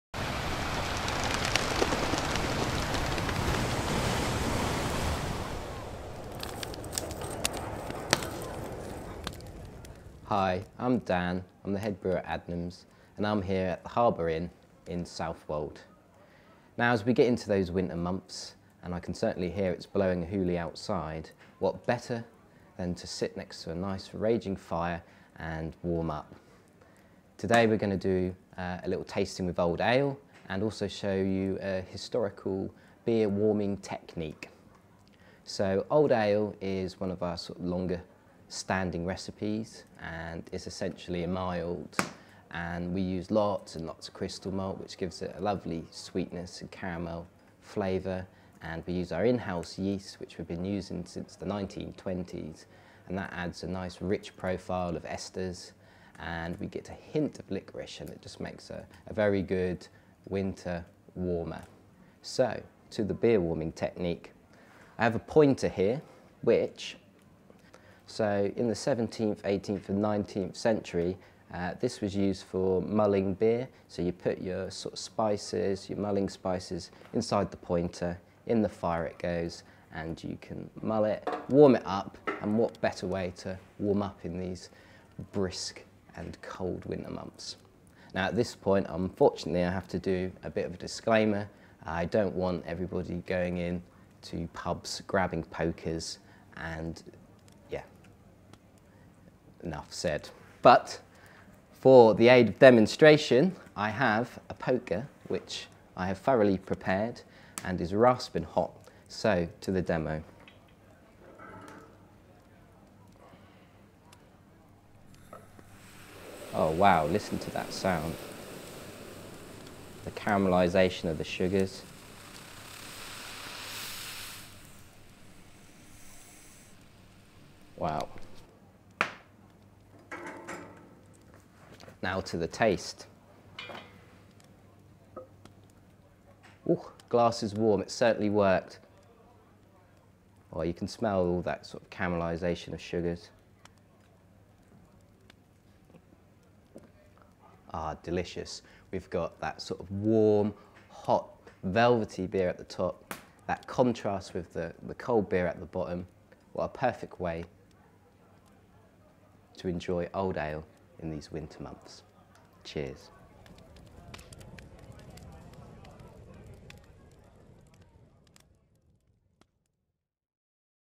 Discover Old Ale
Head Brewer Dan talks you through Old Ale, one of our oldest brews and shares a few of his favourite ways to enjoy a pint! All actions in this video were performed by trained professionals, please do not try this at home.